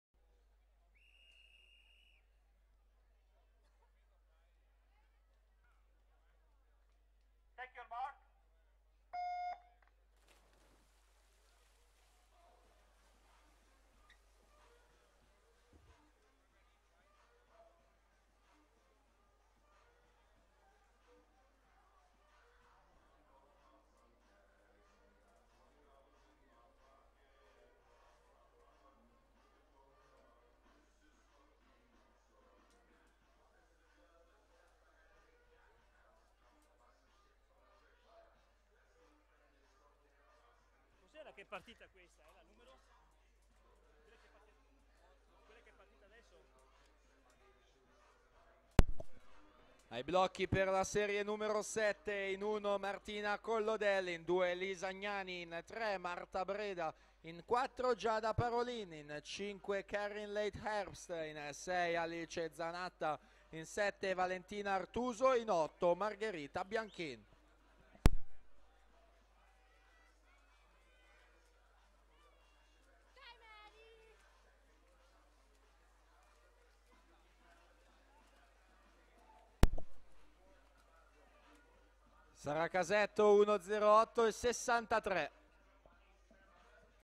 100 Stile Libero Ragazzi Femminile (Serie 8) - Treviso Swim Cup (Swimming)
Atleti partecipanti:
in corsia 1 RADO ARIANNA (2001) della soc. Rappr. HYDROS iscritta con 01:04.04
in corsia 2 ARAPI SABRINA (2001) della soc. Rappr. HYDROS iscritta con 01:03.99
in corsia 3 BRUGNOLI GIORGIA (2002) della soc. FONDAZIONE BENTEGODI iscritta con 01:03.80
in corsia 4 WALLIS REBECCA (2001) della soc. WATERBORN iscritta con 01:03.66
in corsia 5 MENEGUZZO ANDREA (2001) della soc. AQUAREA VICENZA iscritta con 01:03.79
in corsia 6 ROLFE JORDAN-JENNA (2001) della soc. WATERBORN iscritta con 01:03.91
in corsia 7 CANOS CERVERA PALOMA (2002) della soc. ALBATROS iscritta con 01:04.00
in corsia 8 MARCON LISA (2002) della soc. RANAZZURRA S.S.D. iscritta con 01:04.10